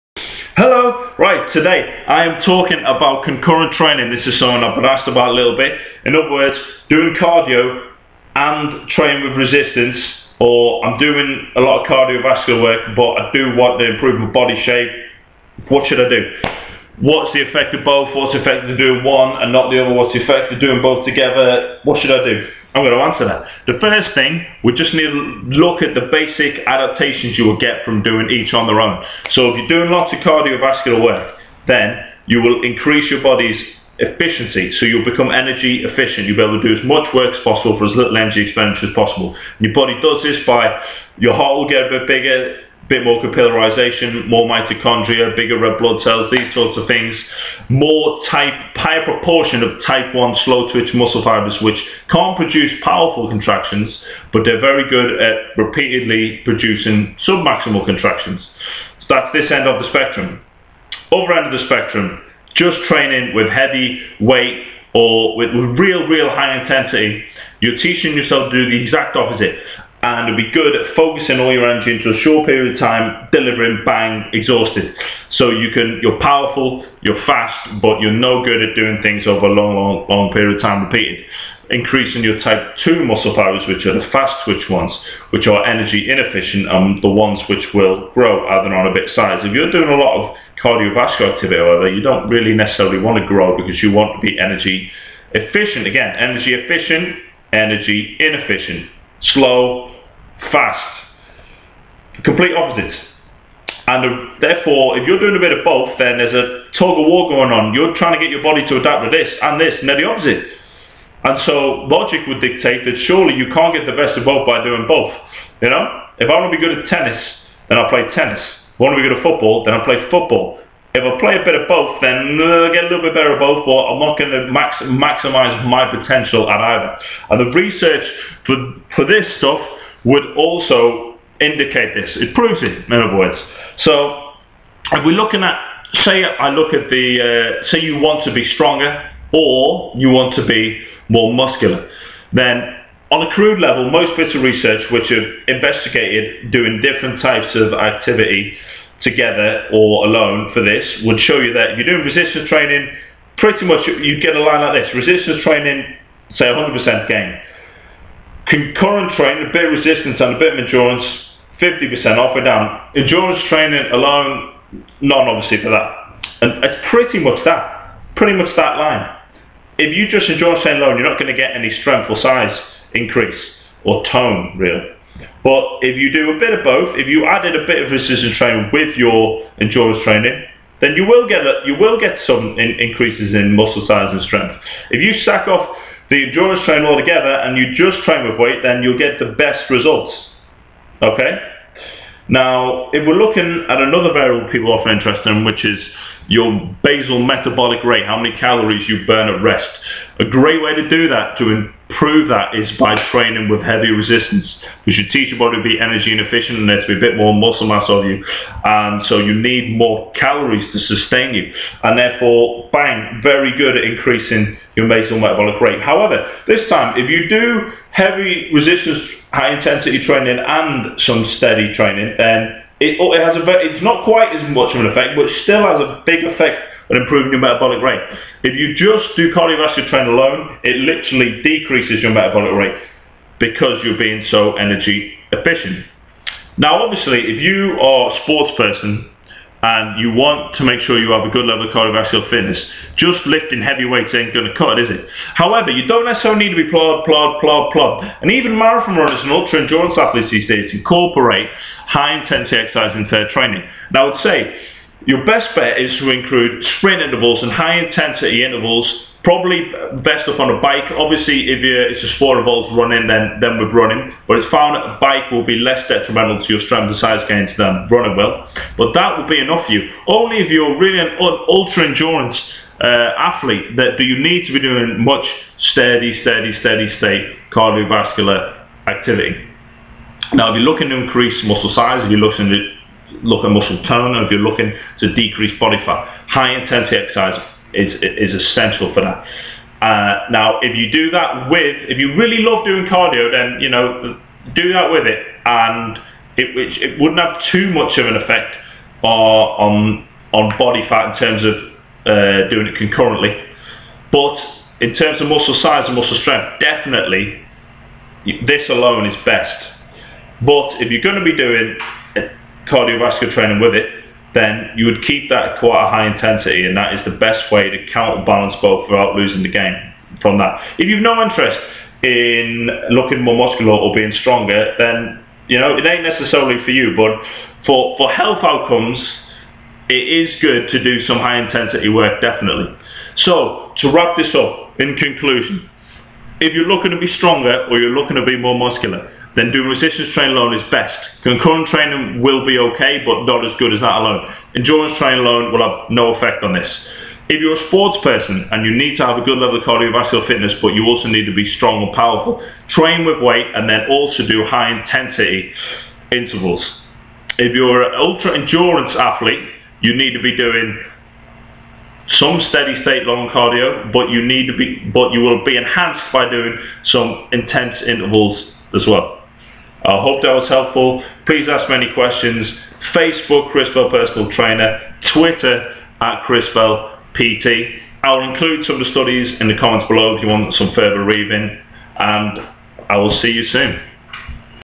Cardio and weight Training together??
An overview of concurrent training, what are the effects of each and both together?